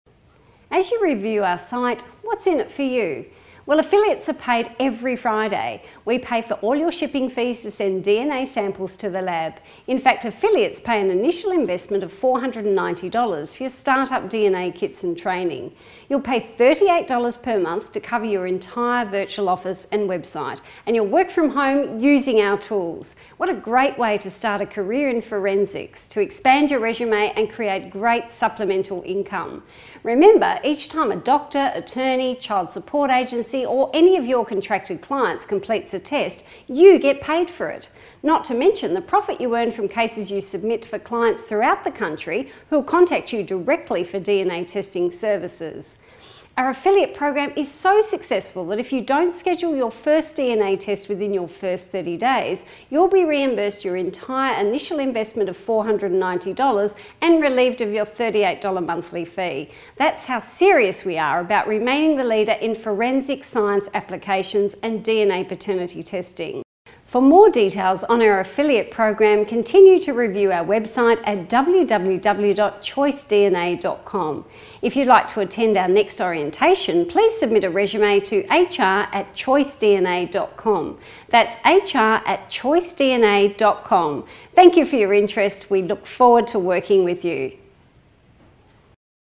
DNA Sample Collector Opportunity - Now Hiring
Choice DNA Laboratory is currently Hiring DNA Sample Collectors. Learn whats required to enter the field of forensics and help families answer difficult questions regarding paternity!